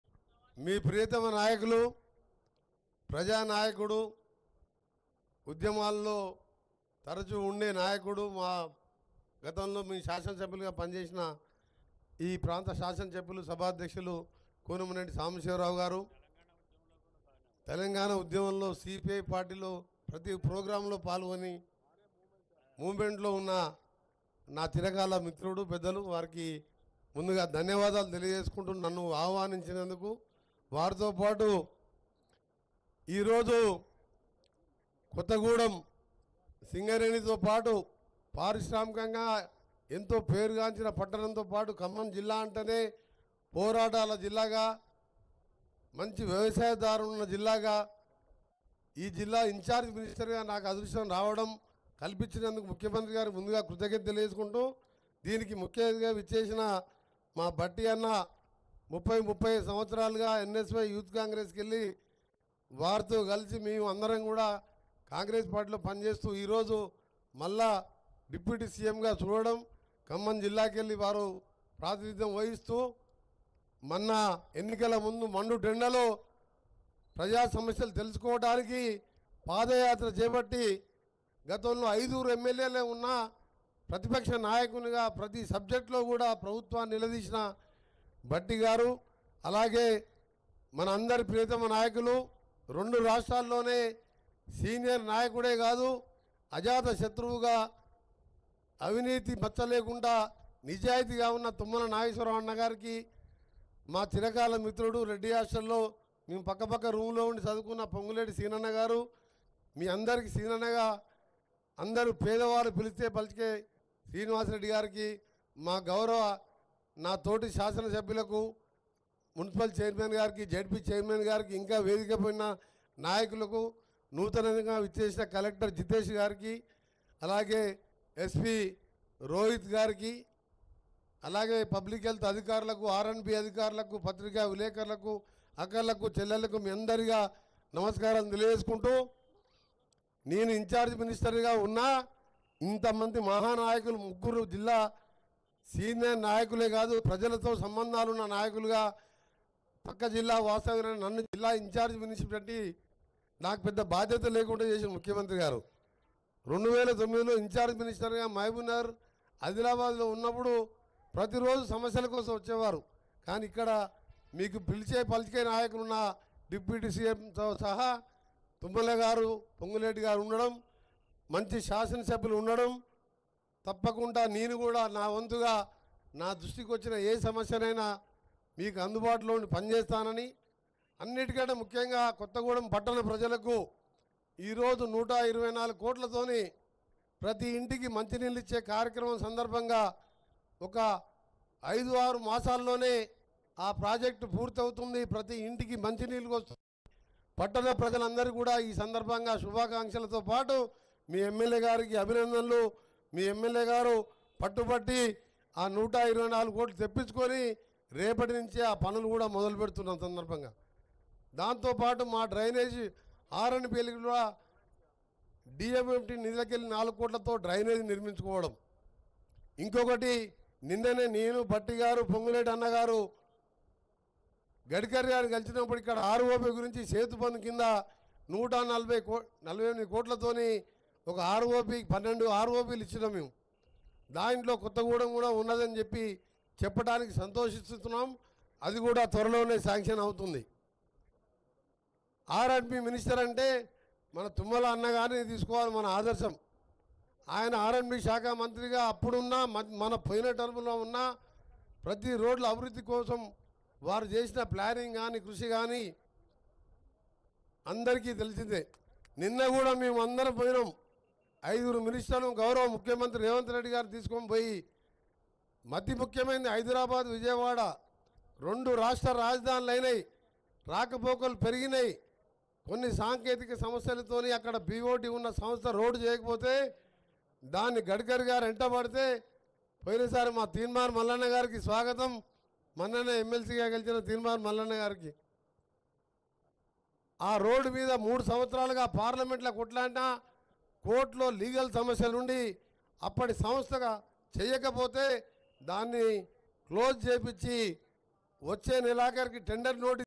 మీకు ఇచ్చిన హామీలే కాదు...కొత్త పతకాలు తీసుకొస్తాం.. | Komati Reddy Venkat Reddy Comments
Watch : LIVE : డిప్యూటీ సీఎం బట్టి విక్రమార్క || Deputy CM Bhatti Vikramarka  @Hittvtelugunews